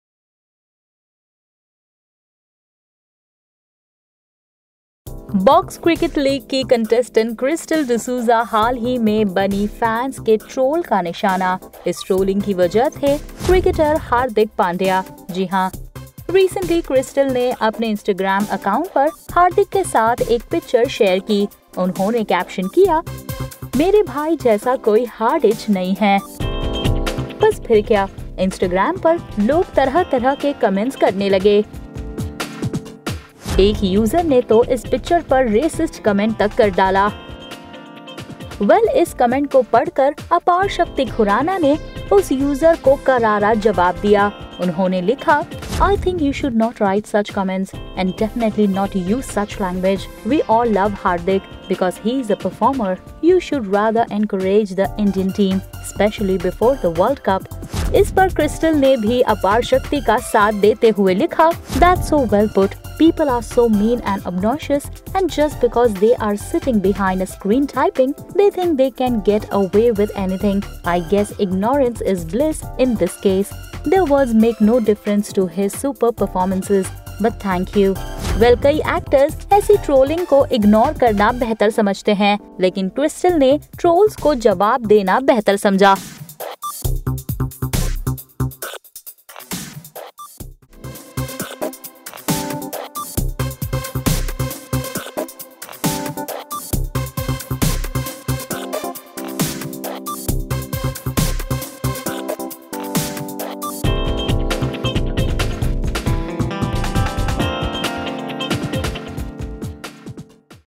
Krystle D'Souza Trolled For Her Selfie With Hardik Pandya
Krystle D'Souza was recently got trolled for clicking a selfie with Indian Cricketer Hardik Pandya. Check out this video.